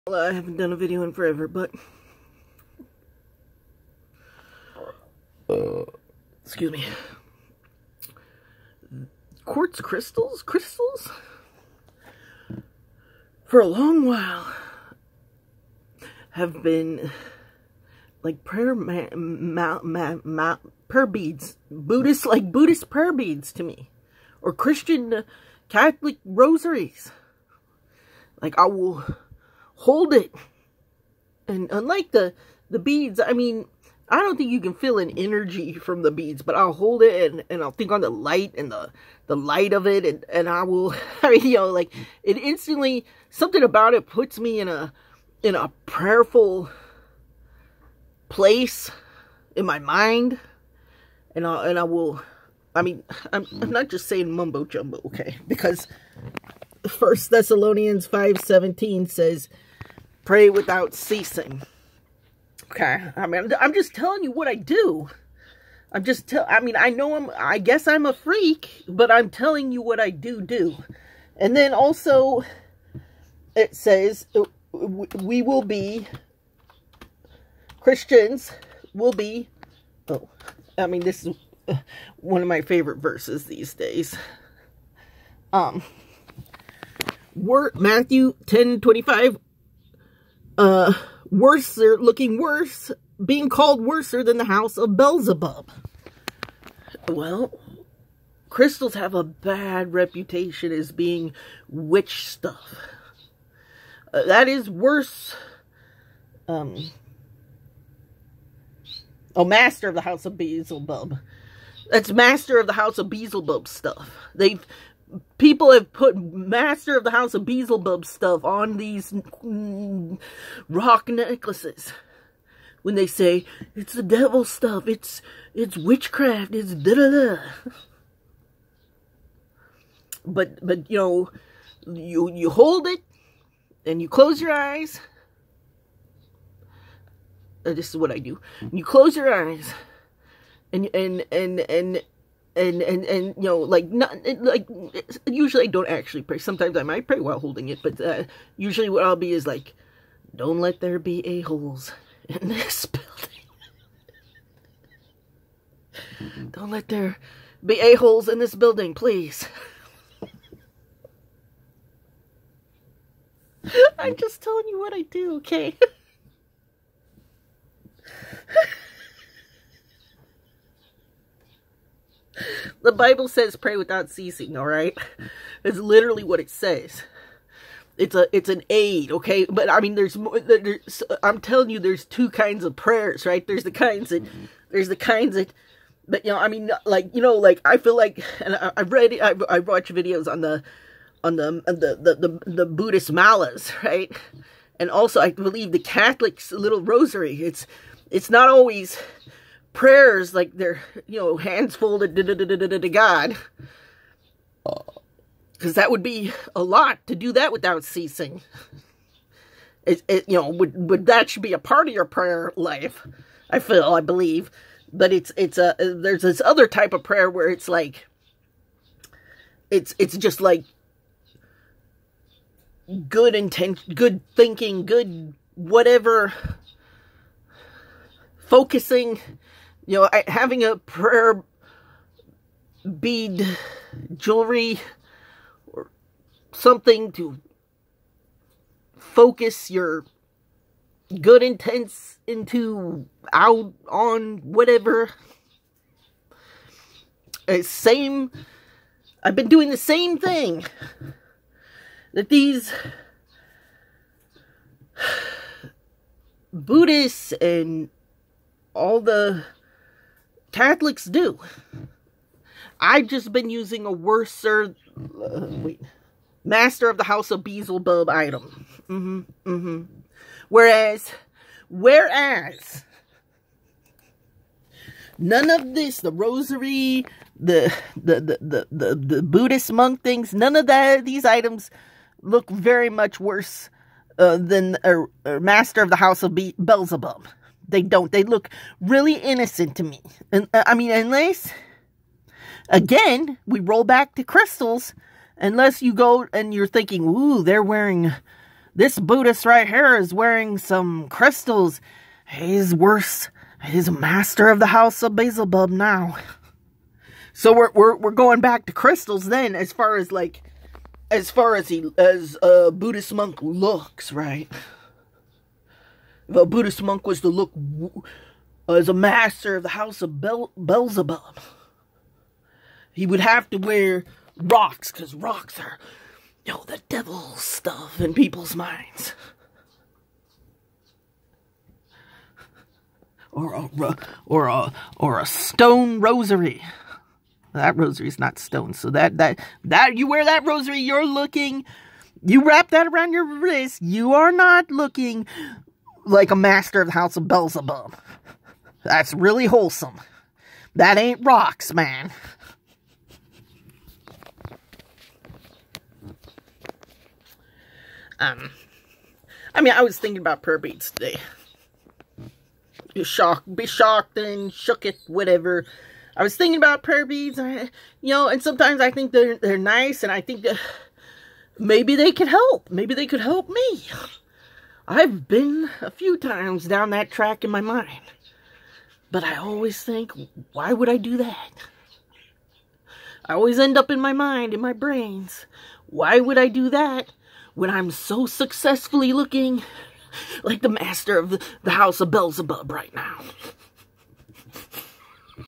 confession I've long used quartz crystal like how malas are used and rosary prayer beads...
praying always its important I believe...to keep your mind in a "hopeful and positive" zone not only for yourself but for everyone else as well. I haven't always always prayed without ceasing... .but am in the best "headspace" when I do pray without ceasing. I've also taken much note that muslims pray 4 times a day. not just 1 prayer. they surround their whole day around prayer. this is best for me to.